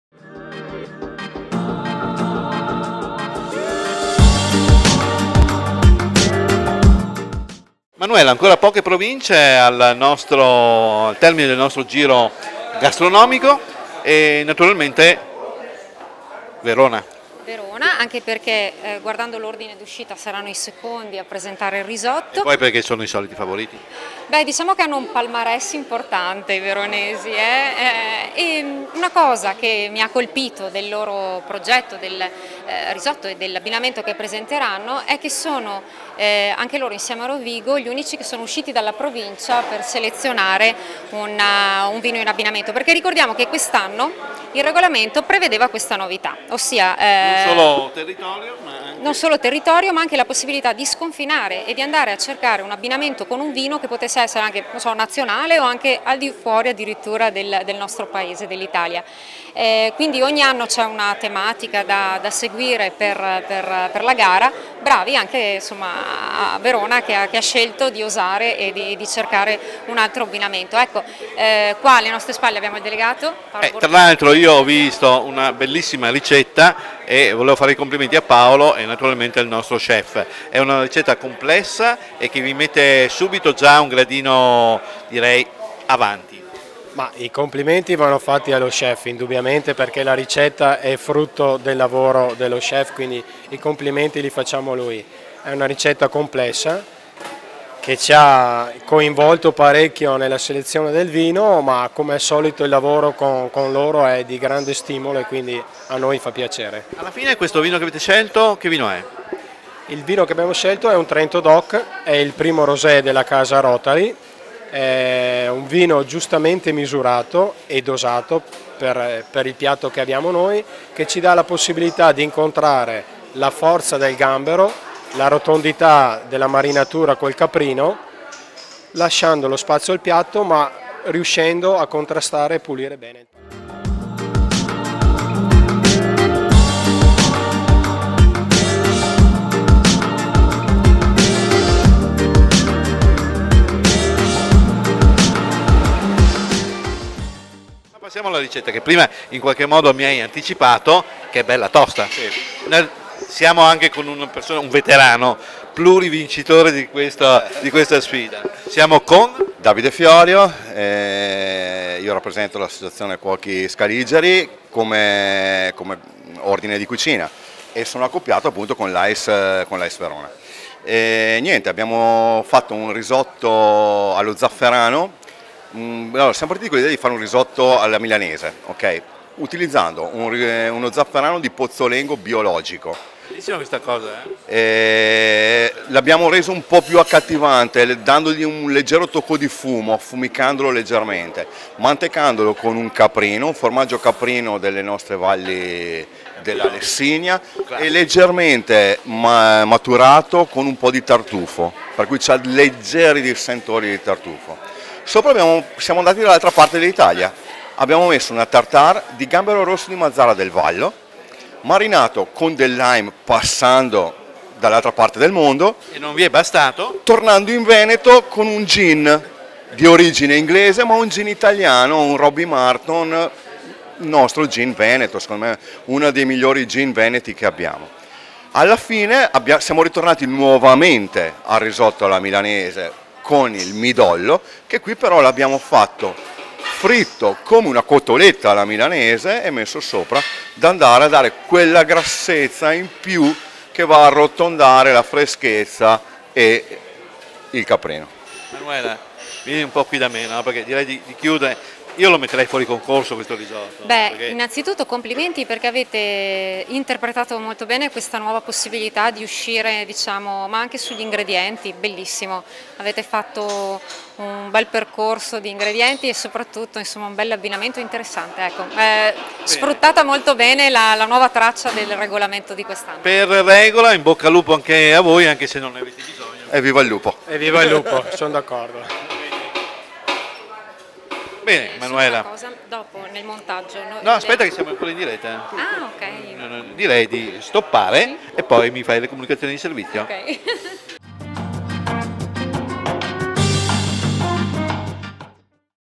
8° Risotto del Sommelier | Ristorante Piper | Verona
Il risotto del Sommelier è il concorso che da otto edizioni premia il miglior abbinamento risotto e vino durante la Fiera del Riso di Isola della Scala.
Il video dedicato al Ristorante Piper | Verona, che ha proposto un Risotto allo zafferano di Pozzolengo mantecato al caprino tartufato e fumo, gambero rosso di Mazara marinato al Roby Martin gin e lime e midollo croccante in abbinamento al vino Alperegis Extra Brut 2013 - Trento Doc - Rotari scelto dai nostri Sommelier AIS Veneto.